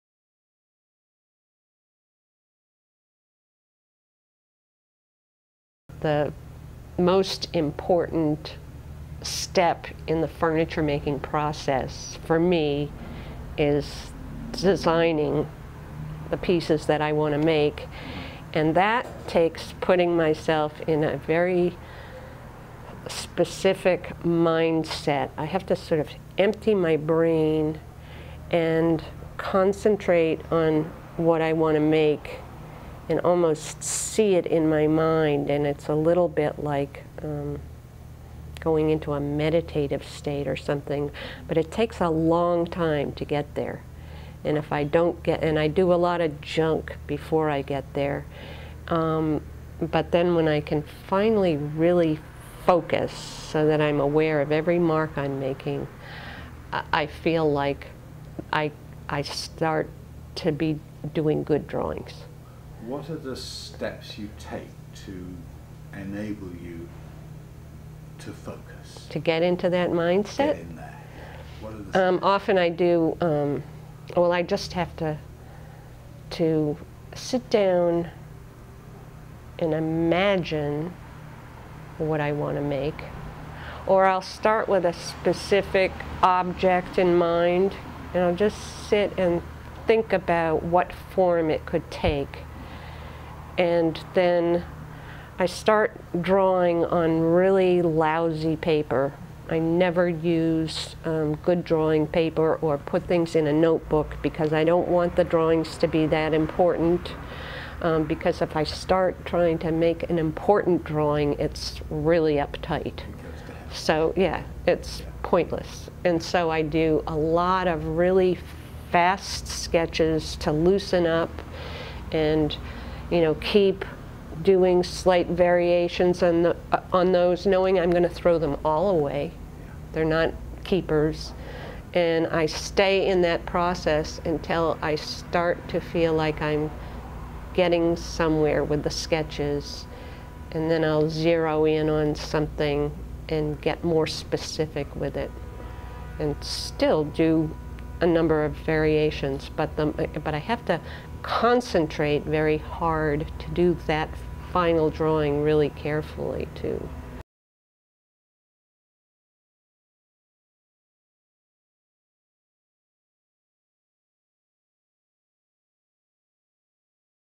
Judy Kensley Mckie on Design Process
A short clip from an interview with renowned furniture designer Judy Kensley Mckie in which she describes the process of developing an idea - a process that, for her, involves a LOT of drawing. Read our article about Judy Kensley McKie here: